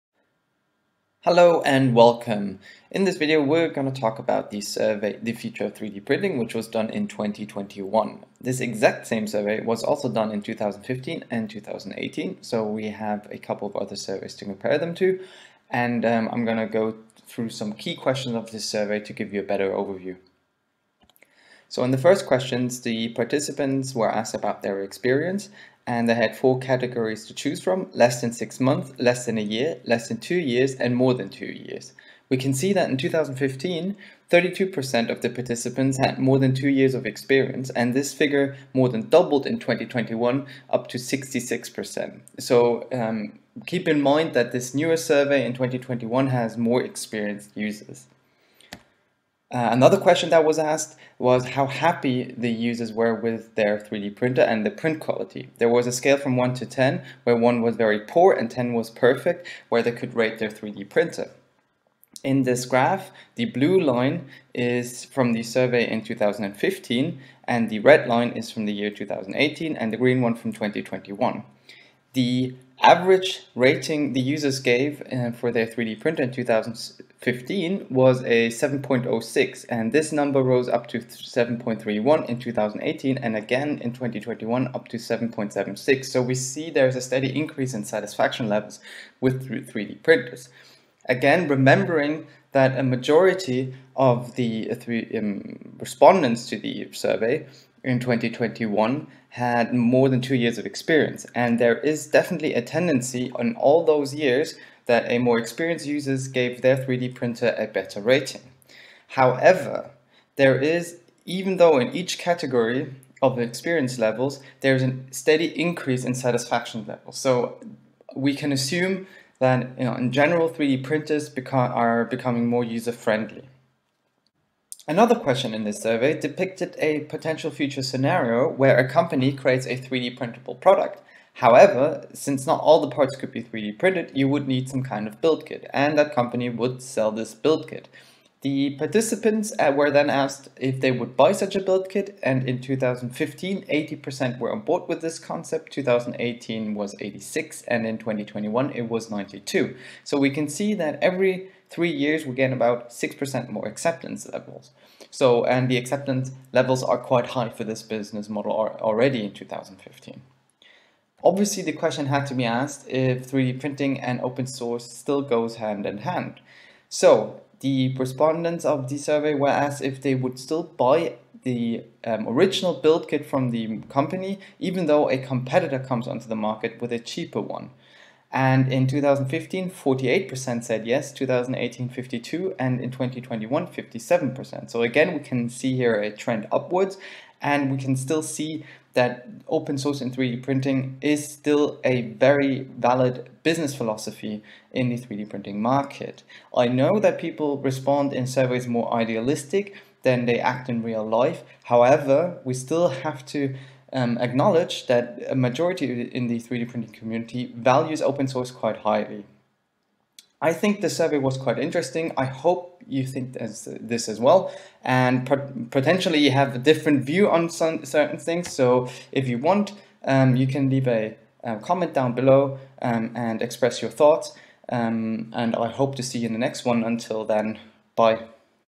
The future of 3D Printing 2021
These are the results of a survey entitled "The future of 3D printing" from June 2021.